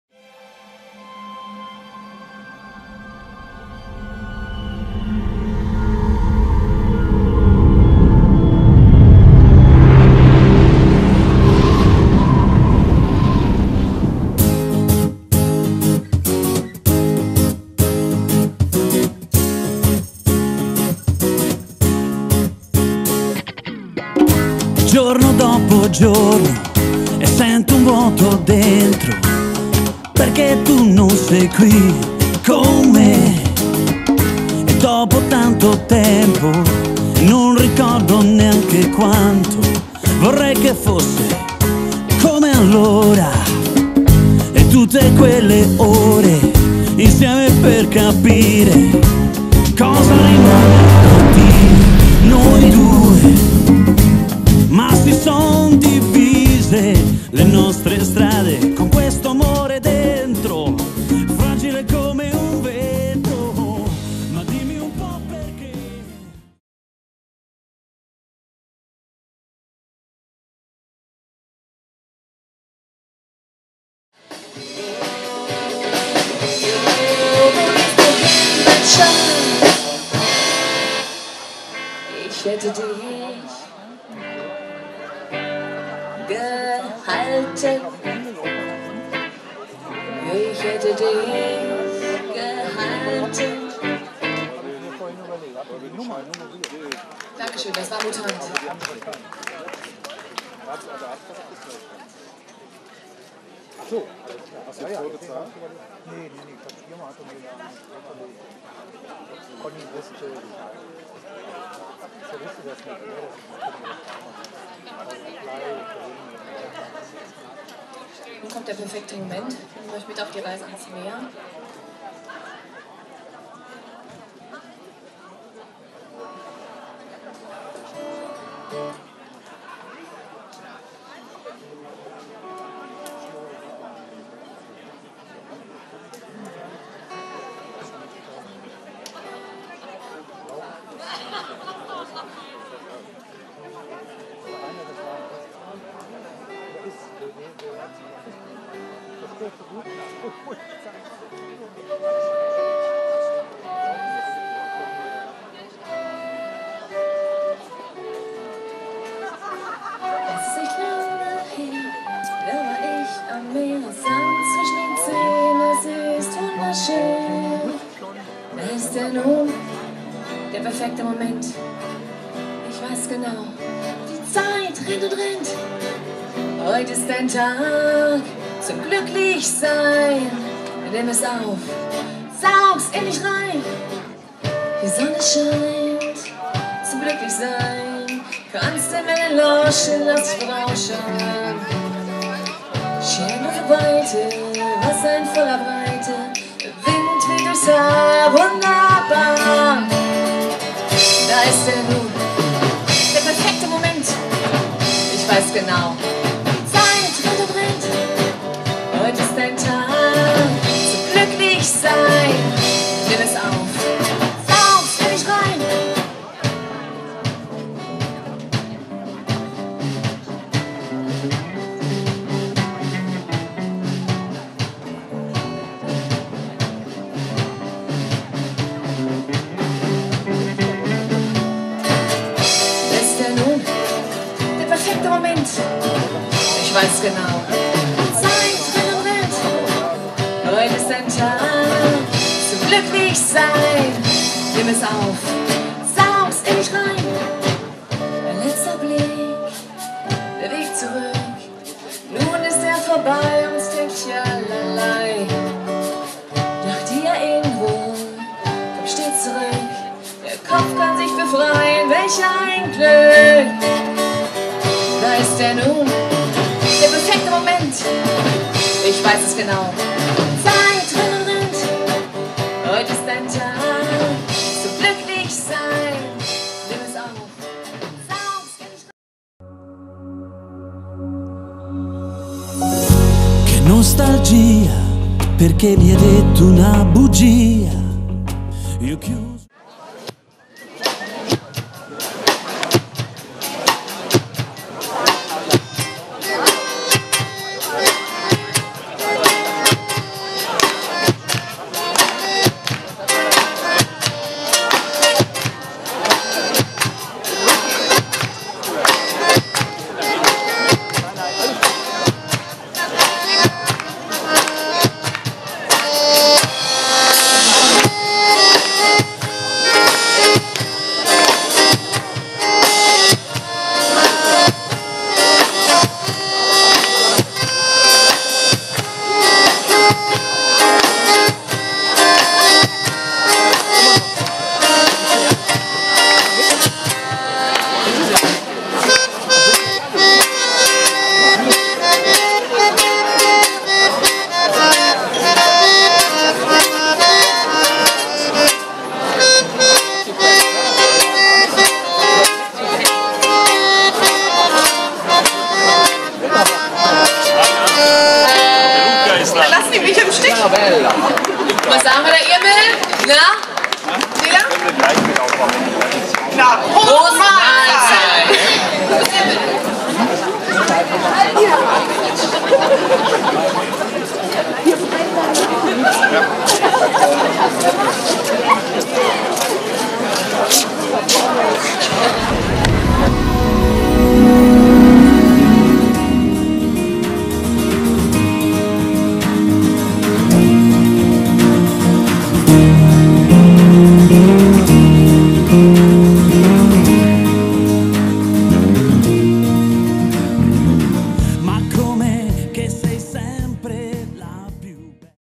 Medusa live in Havixbeck
Auftritt von "Medusa" bei der 8."Italienischen Nacht" im Stift Tilbeck in Havixbeck